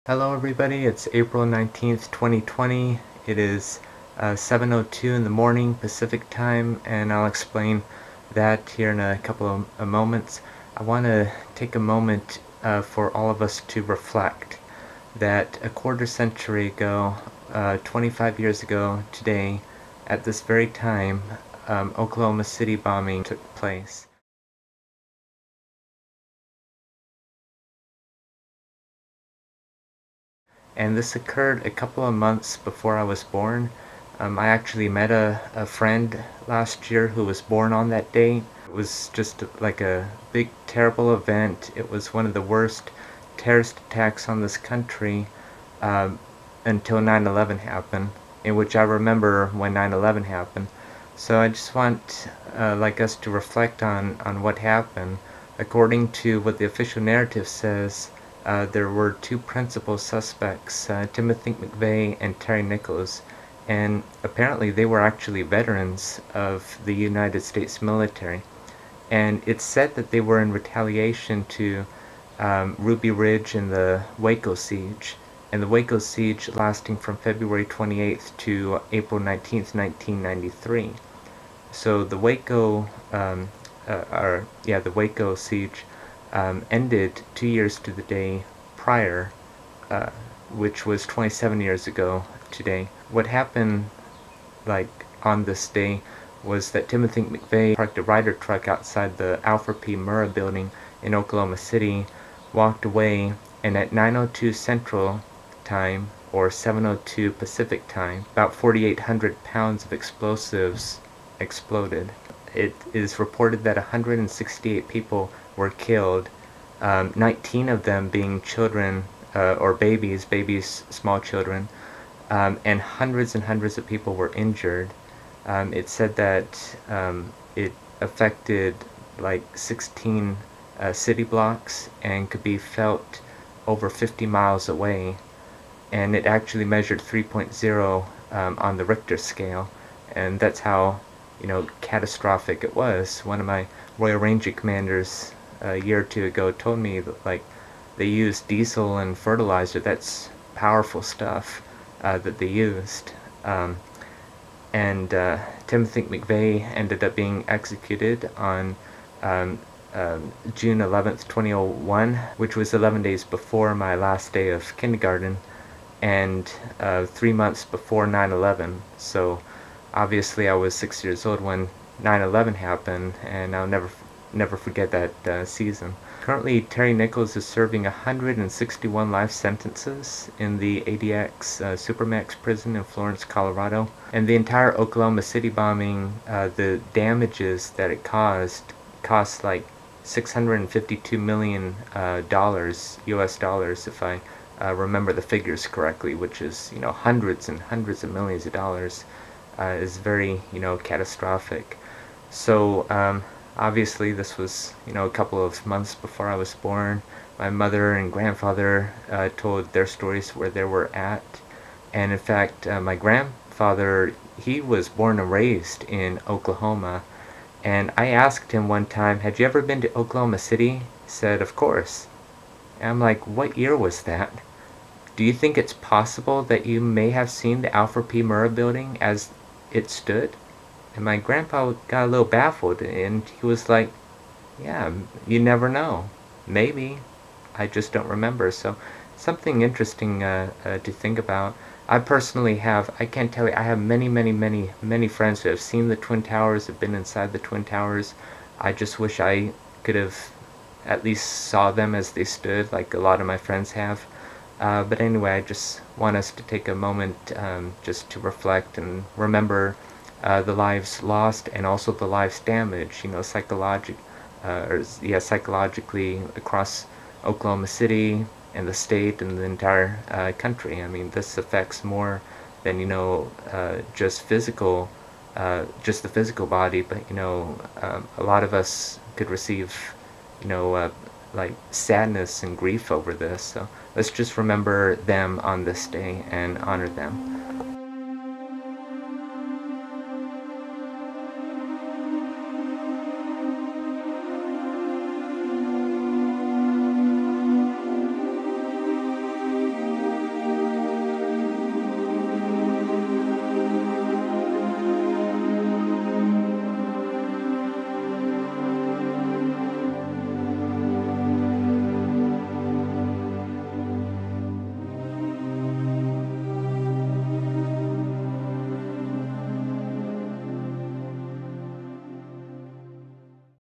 Reflection of the Oklahoma City Bombing, A Quarter Century Later
Filmed in April 19, 2020.
May we never forget and always remember the innocent people who were tragically killed a quarter century ago today, on April 19, 1995, in Oklahoma City, at 9:02am. This event from 25 years ago occurred a few months before I was born.

According to what history tells us, it was a direct retaliation against America for Ruby Ridge in August of 1992 and Waco, TX, which culminated exactly two years to the day prior to the Oklahoma City Bombing; the Waco Siege took place starting from February 28, 1993 and concluding on April 19, 1993 (27 years ago today).

168 lives were taken on this day in 1995, when U.S. Military veterans, Timothy McVeigh and Terry Nichols, senselessly detonated 4,800 pounds of the explosives set off in a Ryder truck parked right outside the Alfred P. Murrah Federal Building. Hundreds of others were injured and hundreds of buildings were impacted within sixteen blocks.

The bombing was felt over fifty miles away and registered a 3.0 on the Richter scale. This domestic terrorist attack costs roughly 652 million dollars in damages and was the hugest attack on America until September 11, 2001.

Timothy McVeigh was executed by lethal injection at his own will/desire on June 11, 2001, which was 11 days before my last day of Kindergarten. Terry Nichols is currently held at the ADX Florence Supermax Prison in Florence, Colorado serving 161 life sentences - the same prison which carries terrorists foreign and domestic, including the Unabomber, Shoe Bomber, Underwear Bomber, Boston Bomber, World Trade Center 1993 Bombers, et al.